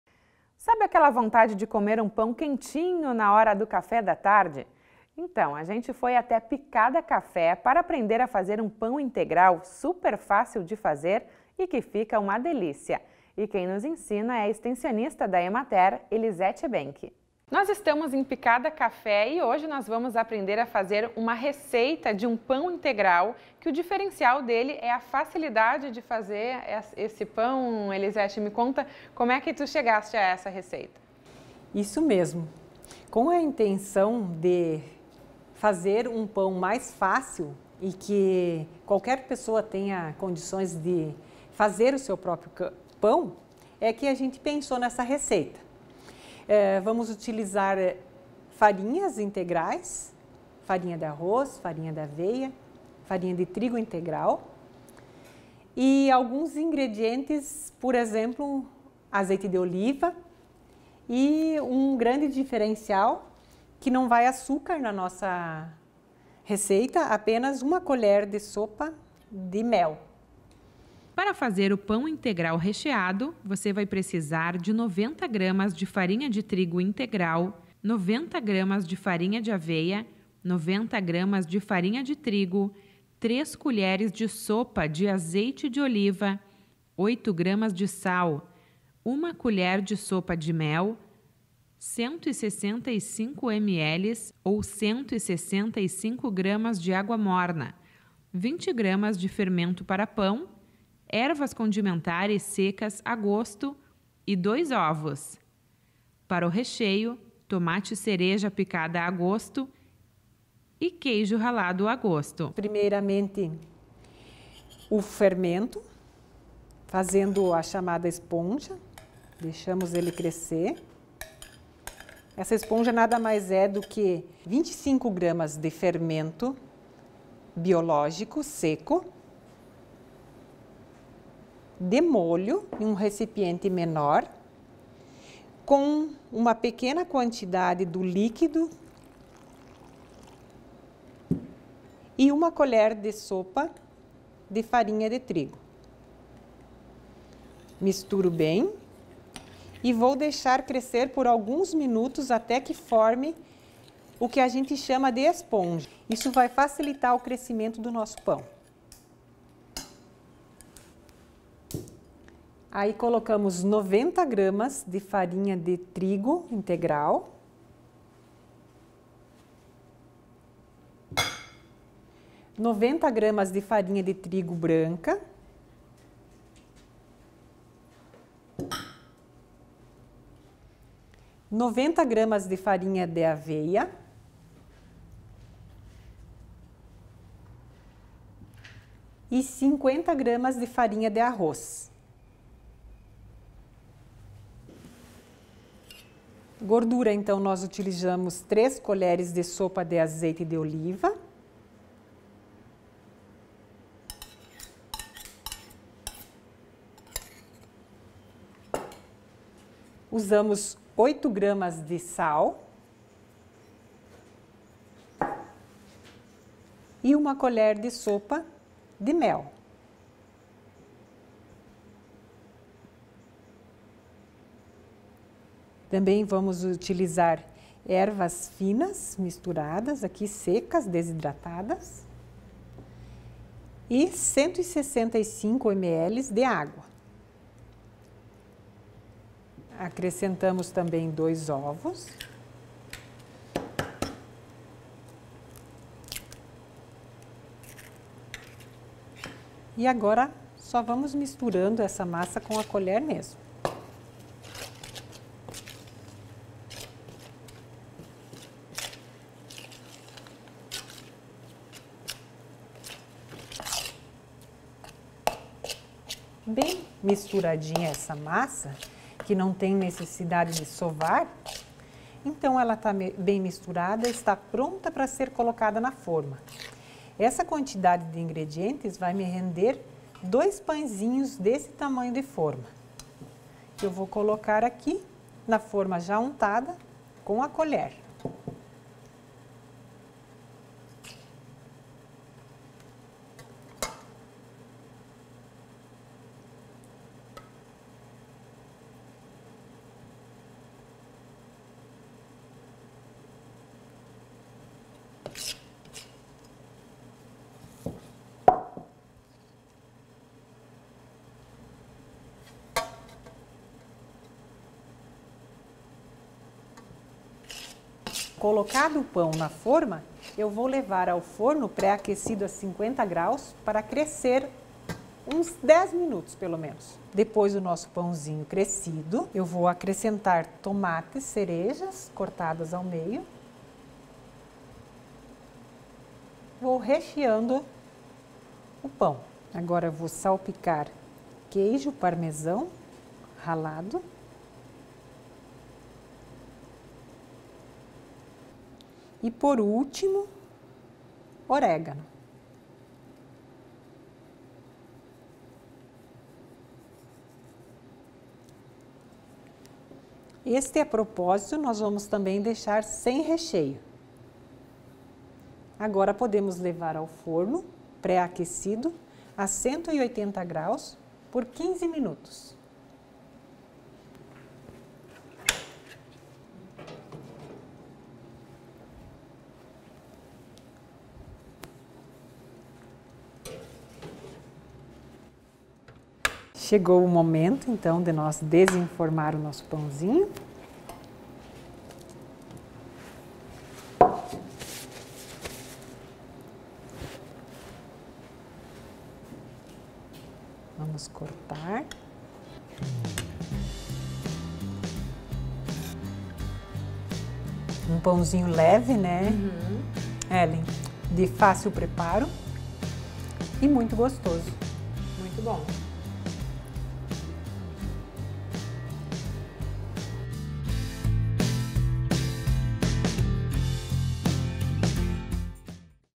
Receita: pão integral - Rio Grande Rural
Sabe aquela vontade de comer um pão quentinho, na hora do café da tarde?! Então, a gente foi até Picada café para aprender a fazer um pão integral, super fácil de fazer e que fica uma delicia!! E quem nos ensina é a extensionista da Emater/RS, Elisete Benke.